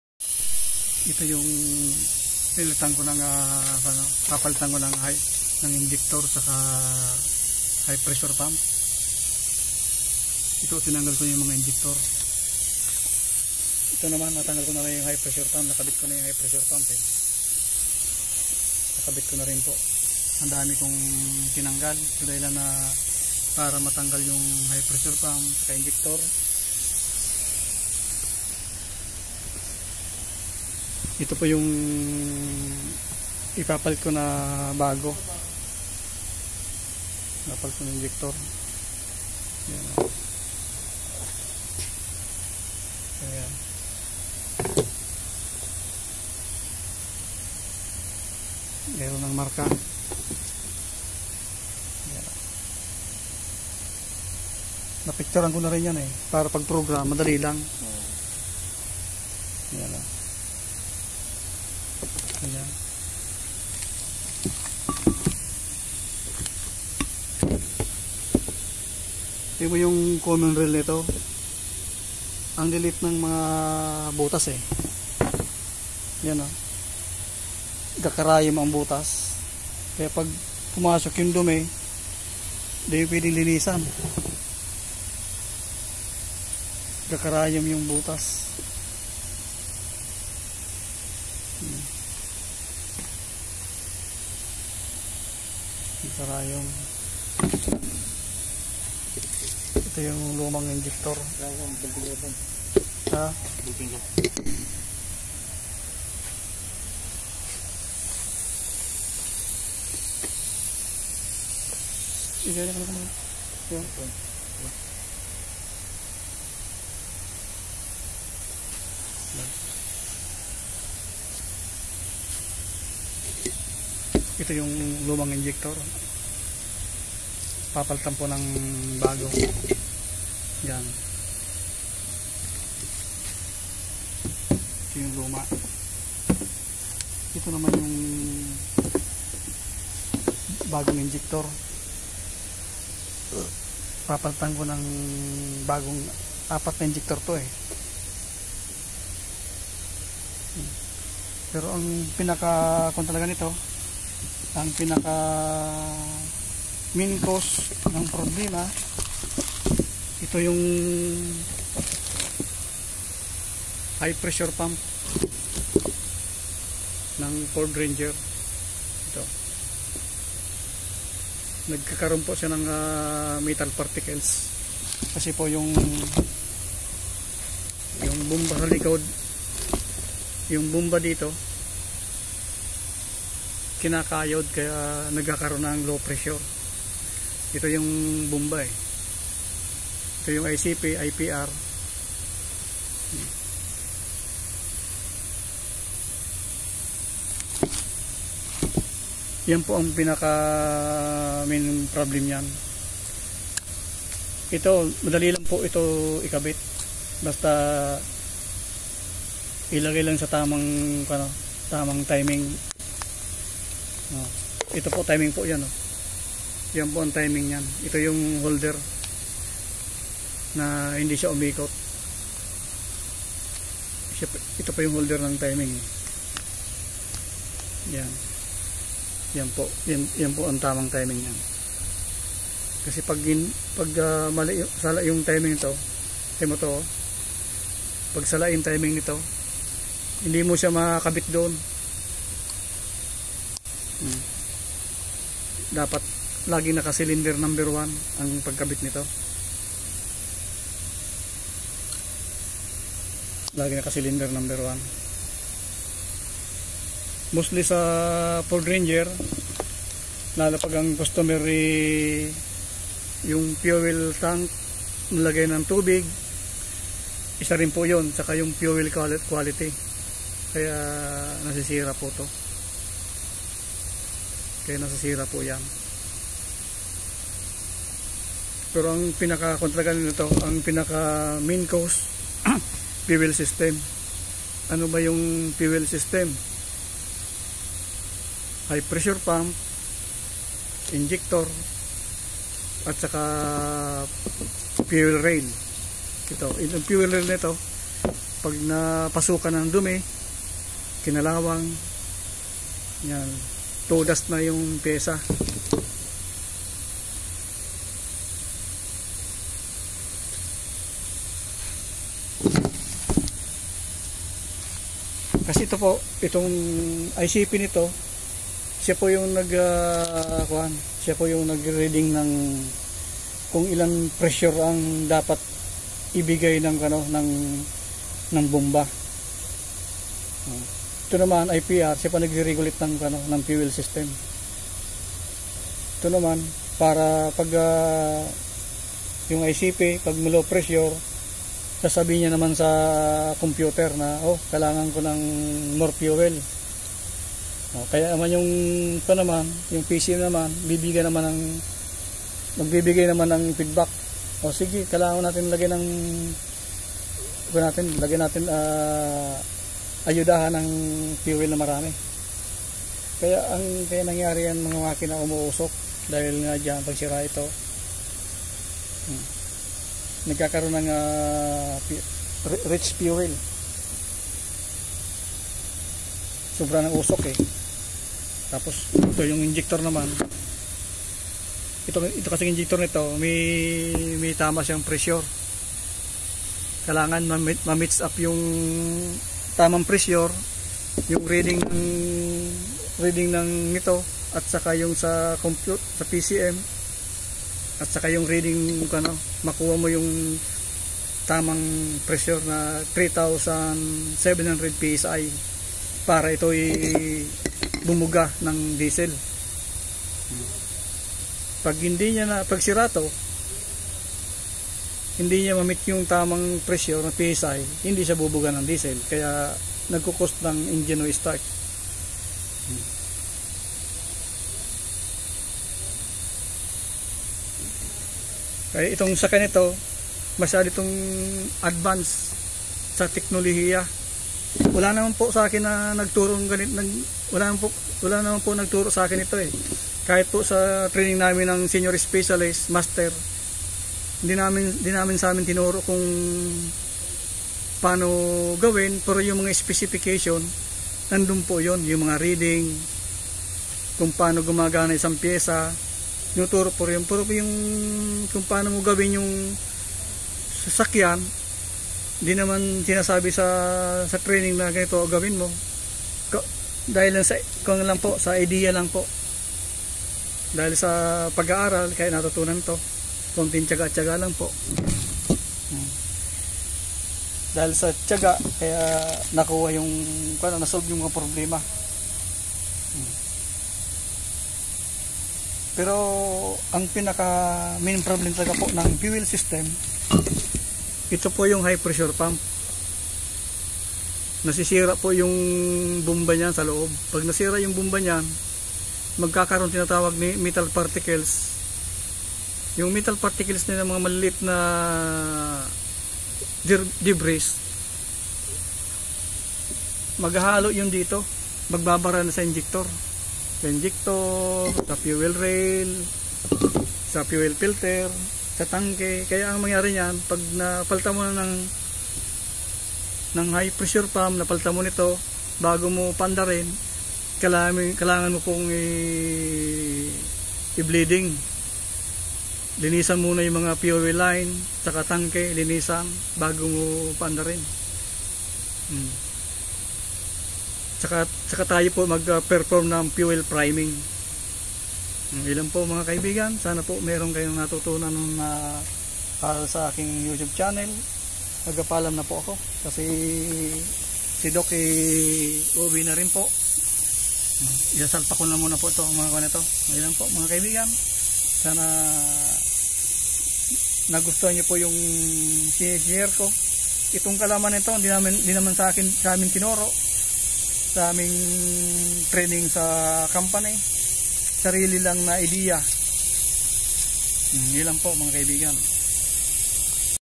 fuel system need replace due to contaminated metal particles engine lack of power
fuel system interna fault